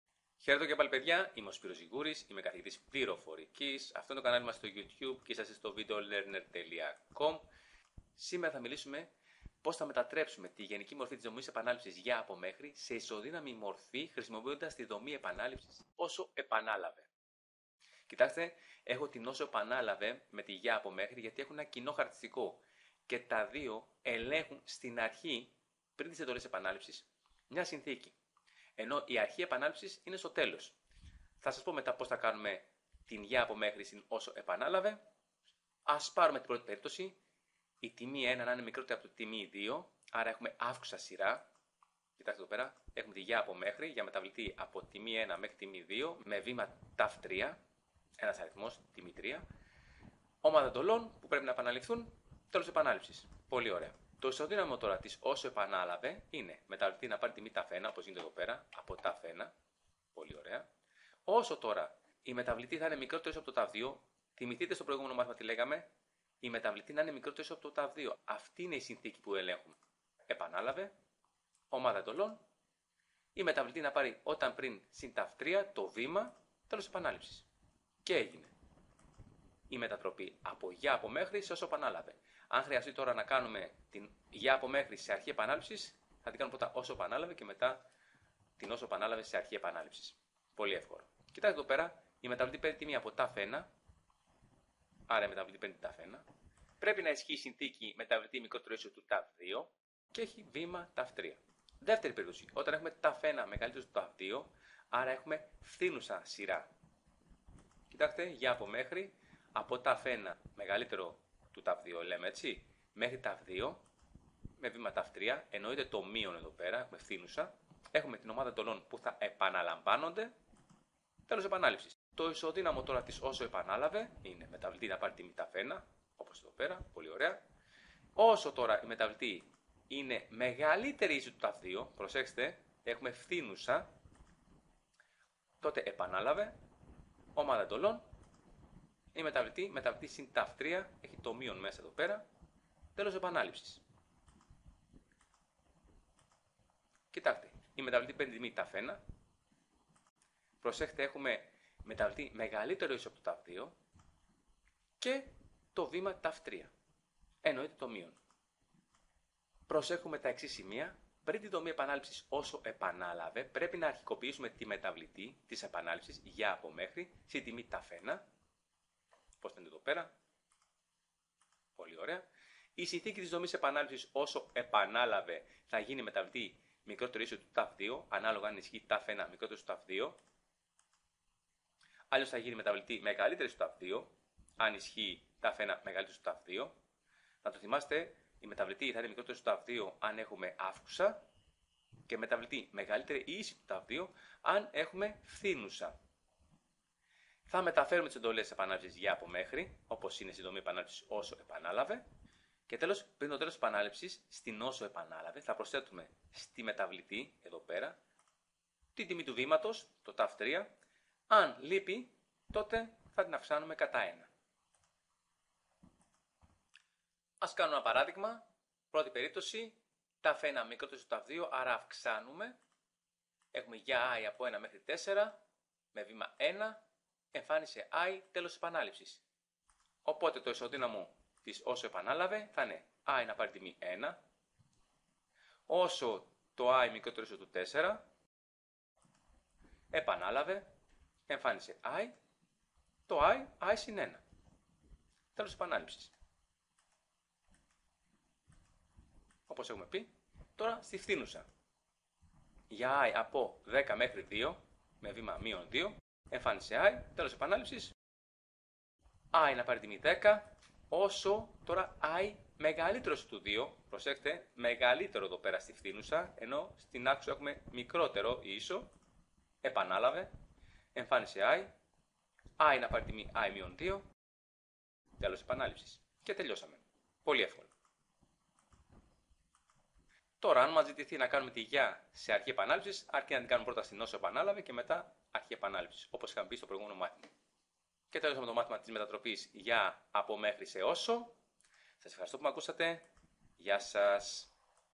Για από Μέχρι -2
Για…από…μέχρι
Να μετατραπεί η γενική μορφή της δομής επανάληψης
Για…από...μέχρι σε ισοδύναμη μορφή χρησιμοποιώντας τη δομή επανάληψης
Όσο….επανάλαβε
1η Περίπτωση τ1≤τ2 (+Αυξάνω)
Προσέχουμε τα εξής σημεία:
1. Πριν την δομή επανάληψης Όσο…επανάλαβε πρέπει να αρχικοποιήσουμε  τη μεταβλητή  της επανάληψης  Για…από..μέχρι  στην τιμή τ1.
2. Η συνθήκη της δομής επανάληψης  Όσο…επανάλαβε
 θα γίνει μεταβλητή≤τ2 (ανάλογα αν ισχύει τ1≤τ2)
 Αλλιώς θα γίνει μεταβλητή≥τ2 (αν ισχύει τ1≥τ2)
3. Θα  μεταφέρουμε τις εντολές επανάληψης  Για…από..μέχρι
    όπως είναι,  στην δομή επανάληψης  Όσο…επανάλαβε
4. Πριν το Τέλος_επανάληψης  στην Όσο…επανάλαβε θα προσθέτουμε στη μεταβλητή   την τιμή του βήματος, αν το  με_βήμα λείπει θα την αυξήσουμε κατά 1.
ΠΡΟΕΤΟΙΜΑΣΙΑ ΠΑΝΕΛΛΗΝΙΩΝ
SPYROS ZYGOURIS
Σπύρος Ζυγούρης
Αναπτυξη Εφαρμογων σε Προγραμματιστικο Περιβαλλον.
Γ Γενικου Λυκειου.
VISUALISED COURSES